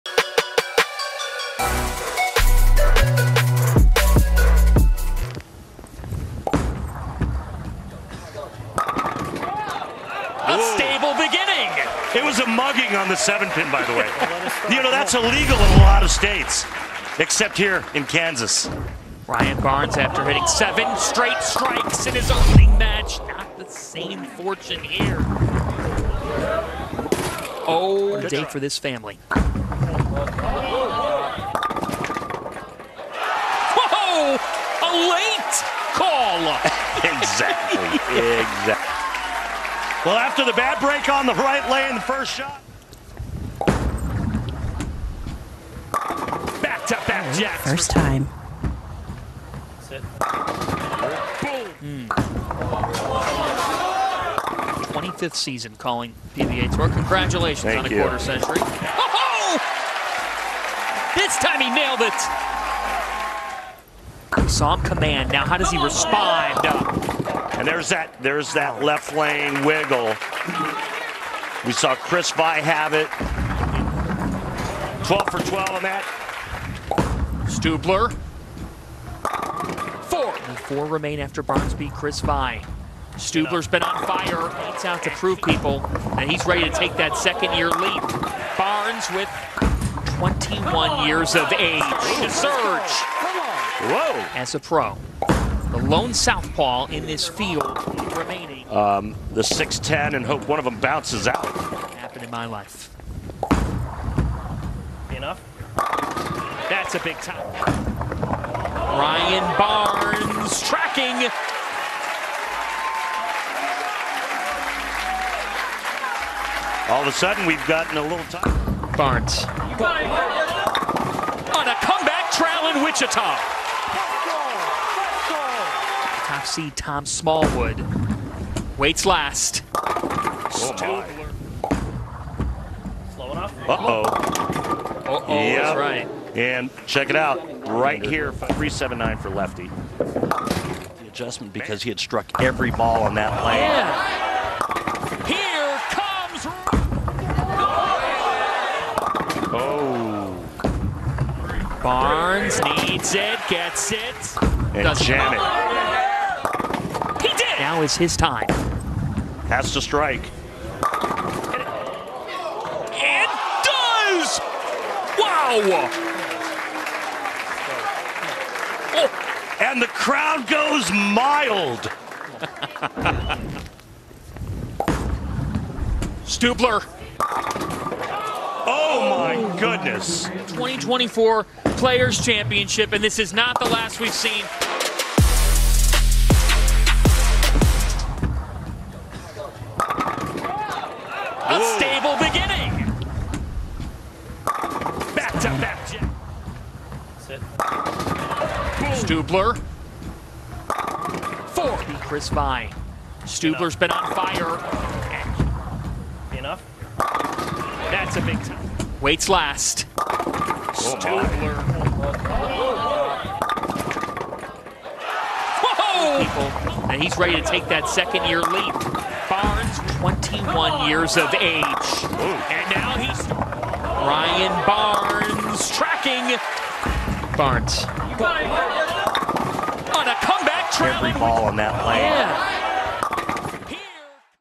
Bowling 2024 PLAYERS MOMENT - GAME2
Chapters:
00:00​​​​​​​​​​​​​​​​​​​​ Introduction

00:05 GAME Analysis

03:04 Analysis after the game

Oil Pattern： Wayne Webb 38
Northrock Lanes
3232 N Rock Rd

Ryan Barnes In the game USE:
【Roto Grip】Tour Dynamix [RG:2.47- Diff:0.026- int. Diff:0.010]
【900 Global】Reality [RG:2.49- Diff:0.052- int. Diff:0.018]
【Storm】Phaze II  [RG:2.48 - Diff:0.051]
【Storm】IQ Tour 78/U  [RG:2.49- Diff:0.029]
Ryan Barnes the game USE: IQ Tour 78/U

Nate Stubler In the game USE:
【Hammer】Black Pearl Urethane  [RG:2.650- Diff:0.015]
【Hammer】NU Blue Hammer [RG:2.586- Diff:0.027]
【Hammer】HAZMAT [RG:2.479- Diff:0.055]
【Brunswick】Mindset [RG:2.487- Diff:0.050- int. Diff:0.021]
Nate Stubler the game USE: Black Pearl Urethane